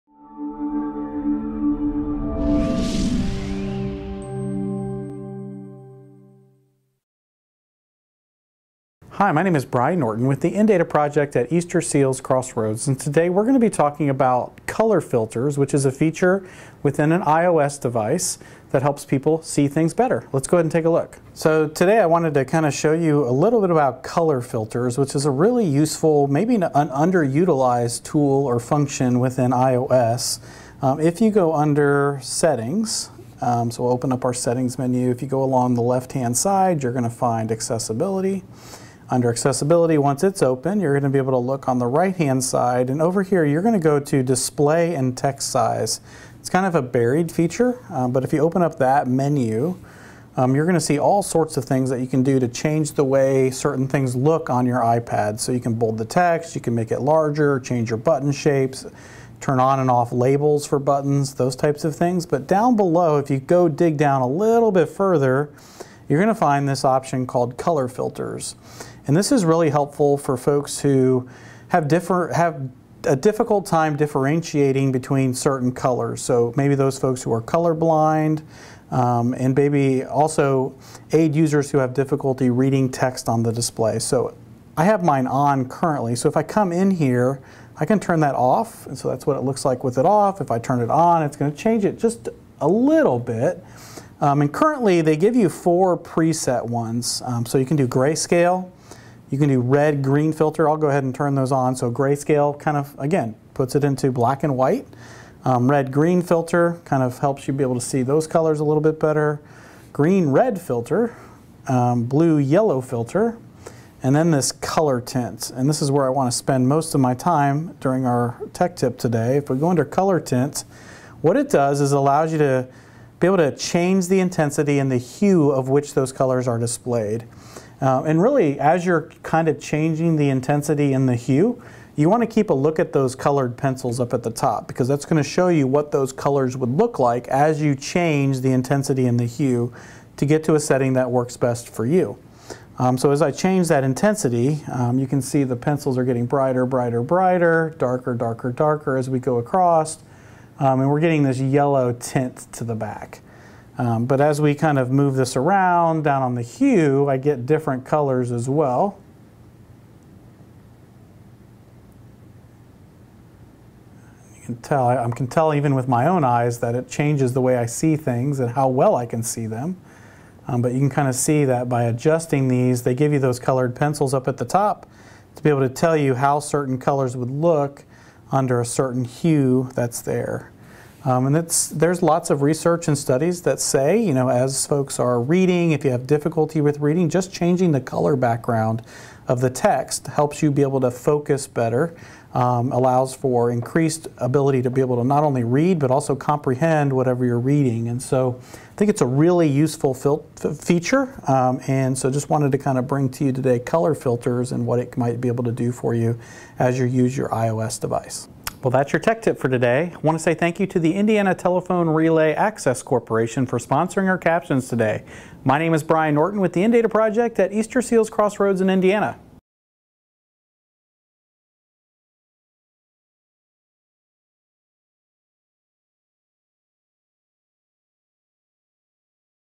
Color Filters on iPhone & iPad
Brian Norton, Director of Assistive Technology, at Easterseals Crossroads shows us Color Filter, which helps you differentiate between colors if you have a visually impairment or are color blind.